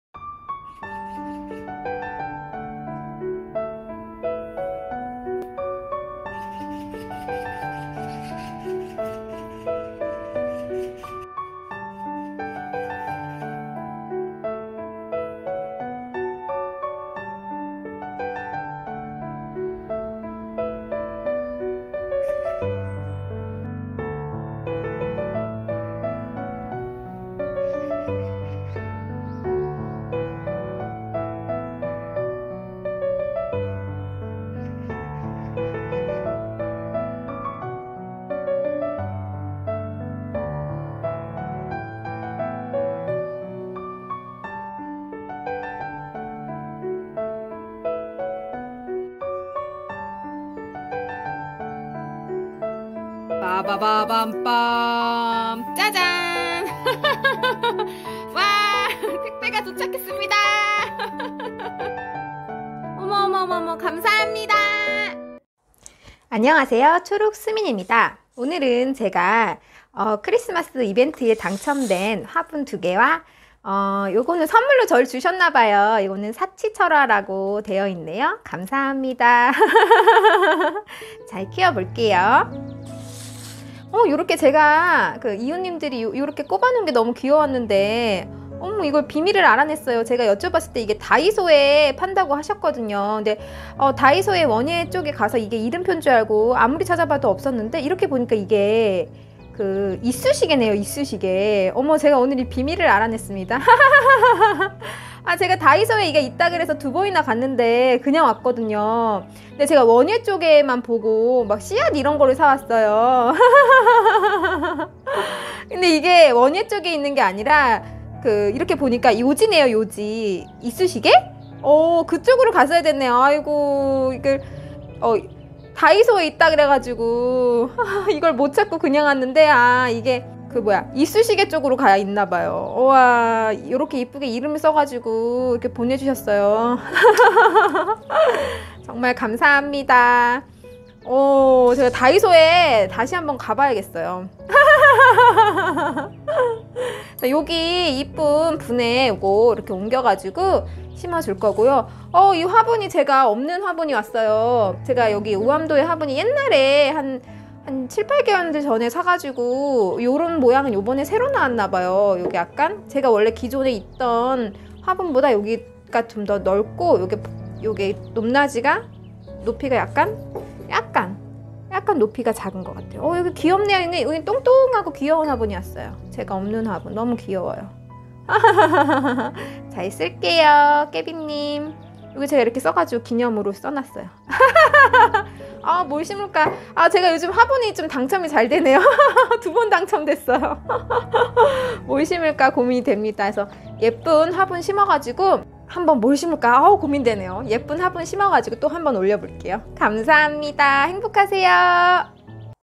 깨비다육 이벤트당첨 ♡ 아침일찍 다육이 밖으로 바람통하게 해주고요 ♡ 택배가 왔어요 succulent 사치철화 우암도예 화분 당첨 됐어요
크리스마스 깨비다육 이벤트 당첨^^

우암도예 화분2개요ㅎㅎ

사치철화는 사랑인가요 ㅎㅎ

즐겁고 행복한 연말입니다